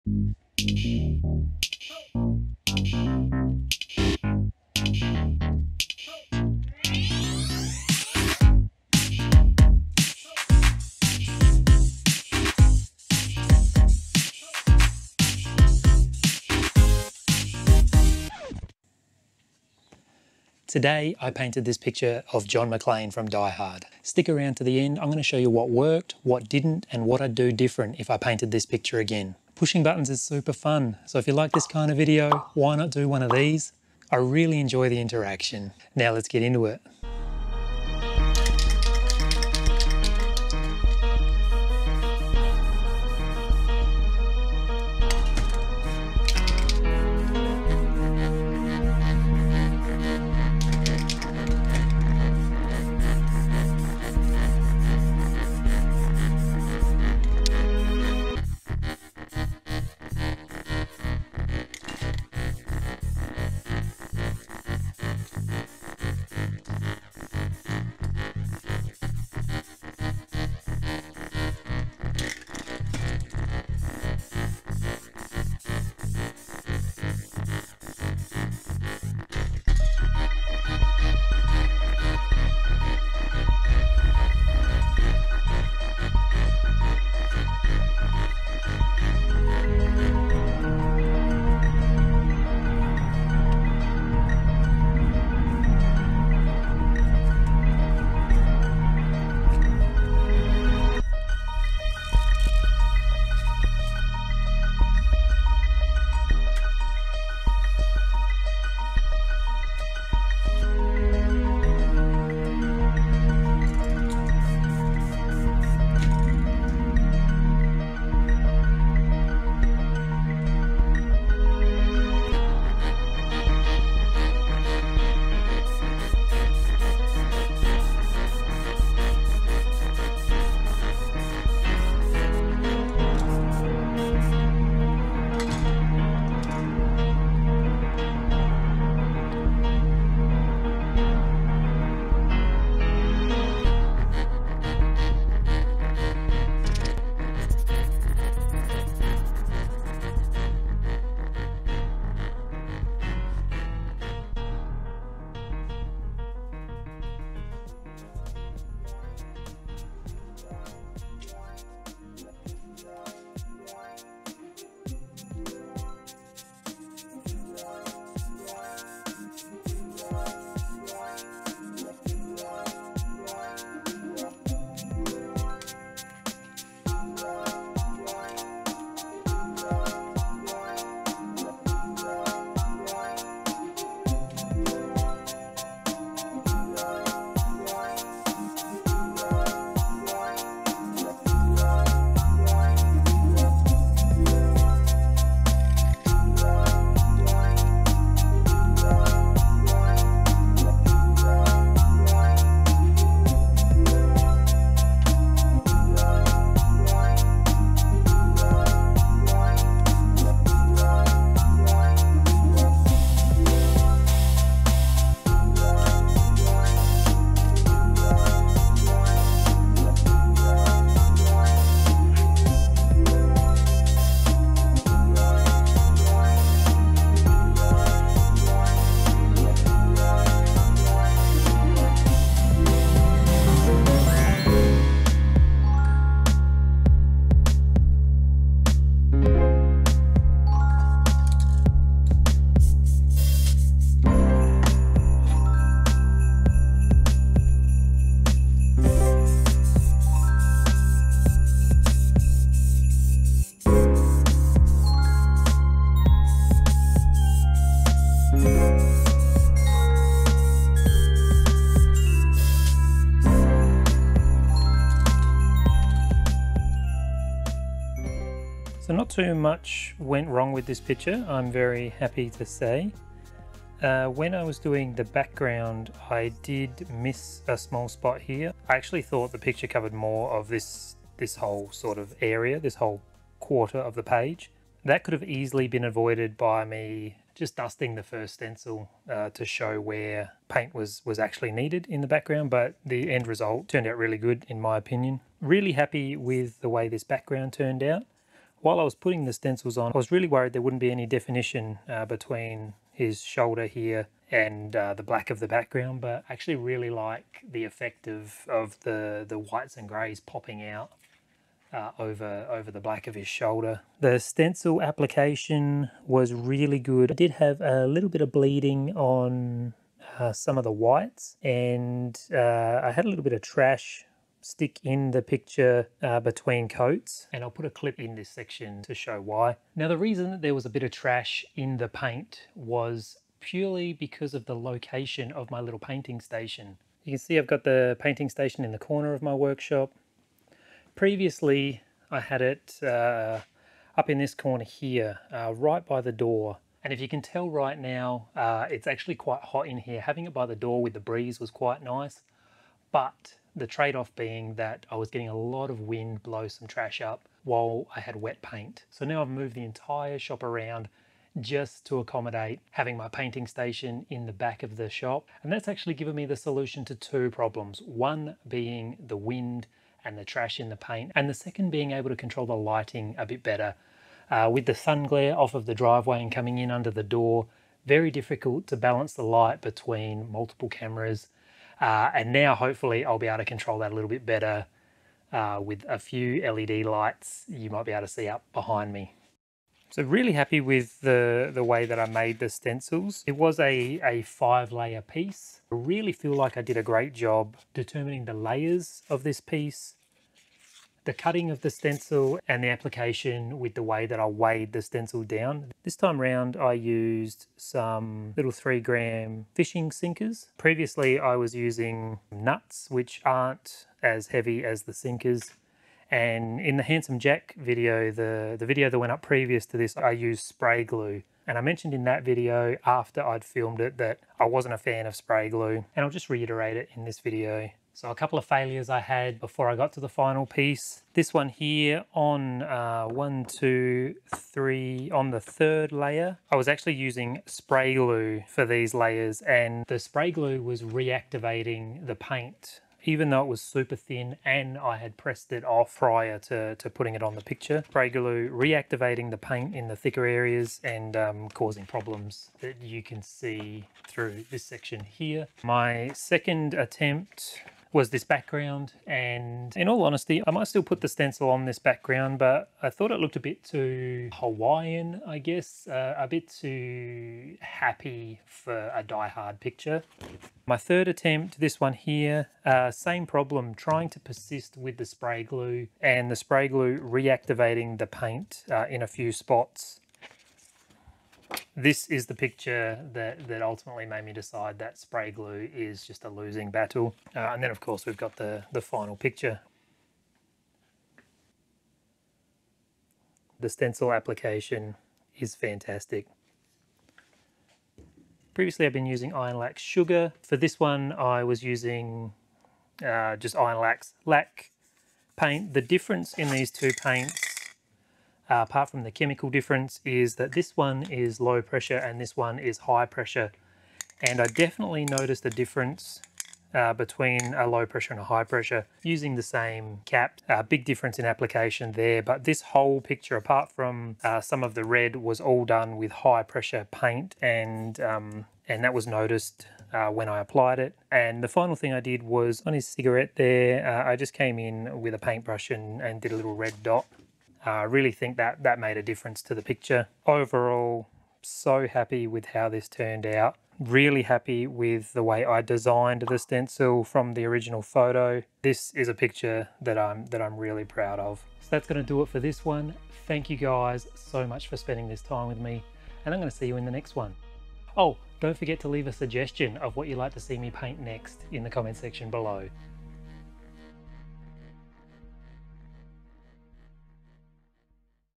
John McLane from DIE HARD - Spray Paint Stencil Art
Christmas is upon us...
Well is was when I started this stencil, turned out pretty awesome.

I've discovered something awesome in spray paint art, there's just something about the technicality of the stencil designs and the freedom of the spray painting really captivates me!